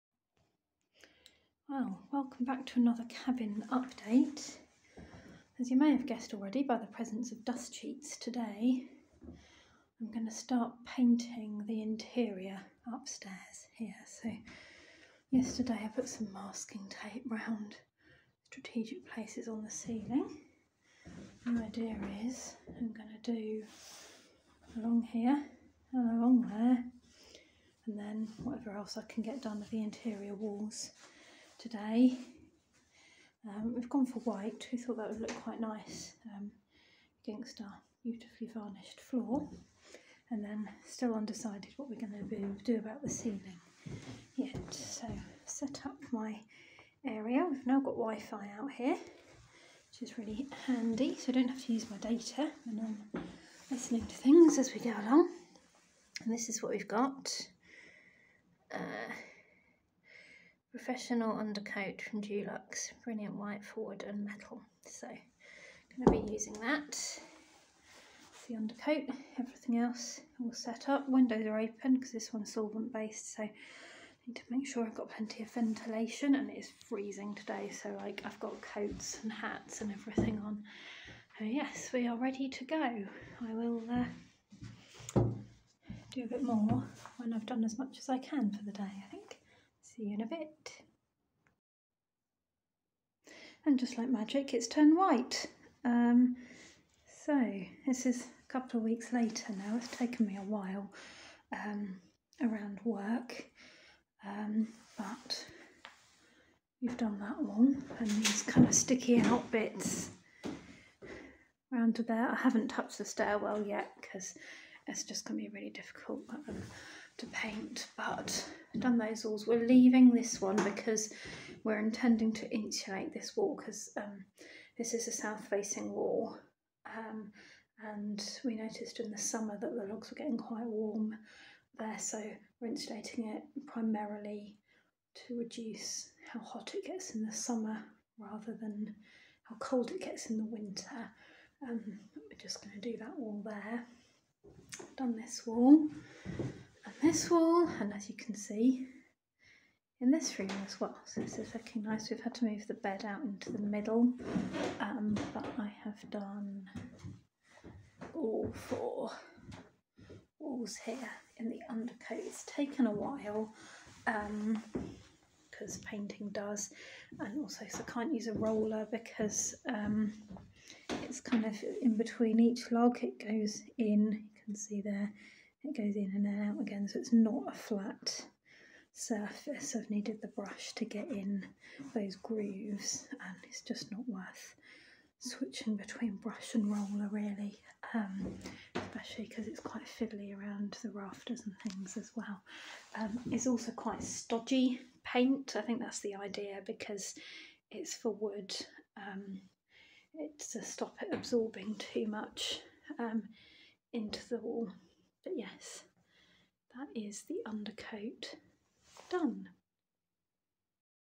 Interior log cabin painting Part 1: the undercoat upstairs
In this video I paint the undercoat on all the walls upstairs. I used a Dulux brilliant white undercoat specifically for wood and metal. Took about 4 days over the course of three weeks.
The next video will be adding a matt emulsion over the top.